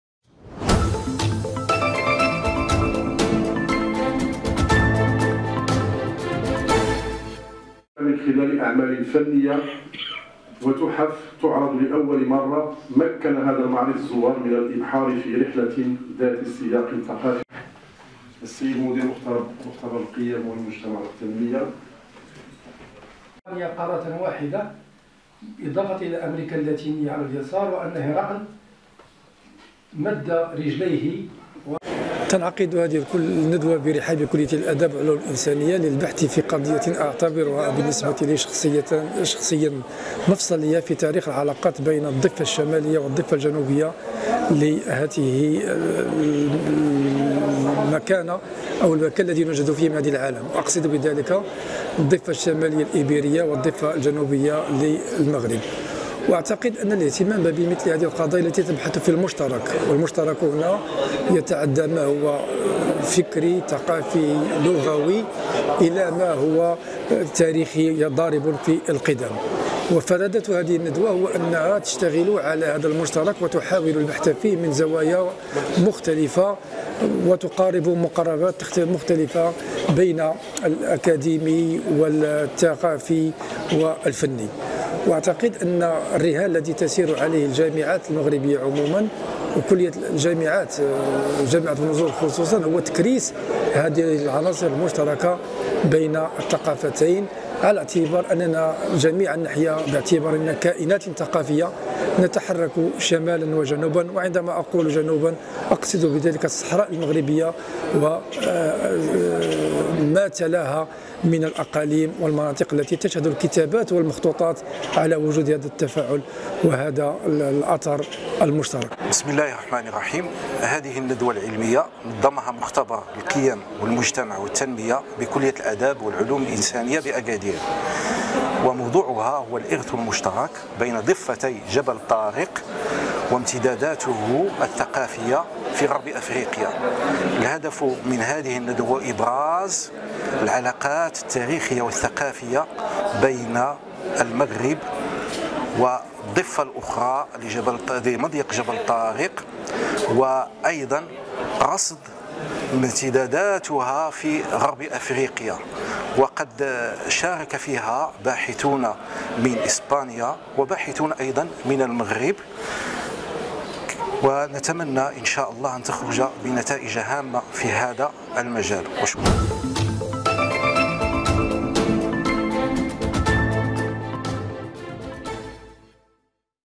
باحثون مغاربة وإسبّان يناقشون الإرث المشترك بين ضفّتي جبل طارق
بين المغرب واسبانيا العديد من الإرث المشترك، في هذا الفيديو يتم التركيز على هذا الإرث الذي يربط بين ضفتي جبل طارق